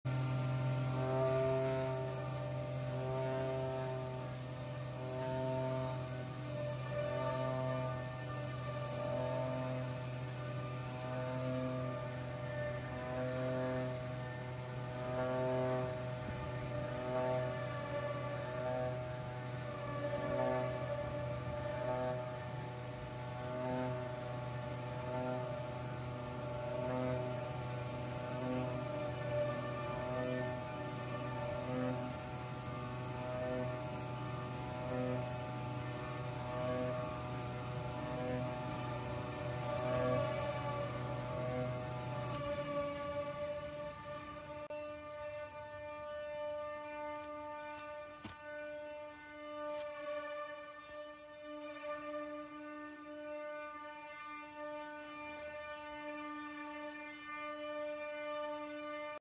EPE Foam Sheet Digital CNC Cutting Machine
Today I would like to introduce a new foam cutting machine. The foam CNC cutting machine is a oscillating knife cutting machine. The EPE foam cutting machine is a high-frequency vibration blade. There will be no dust or powder, and there will be no problems such as hot cutting smoke. In addition, the foam sheet cutting machine is a CNC cutting machine. It cuts according to CAD graphics and no longer needs to make molds.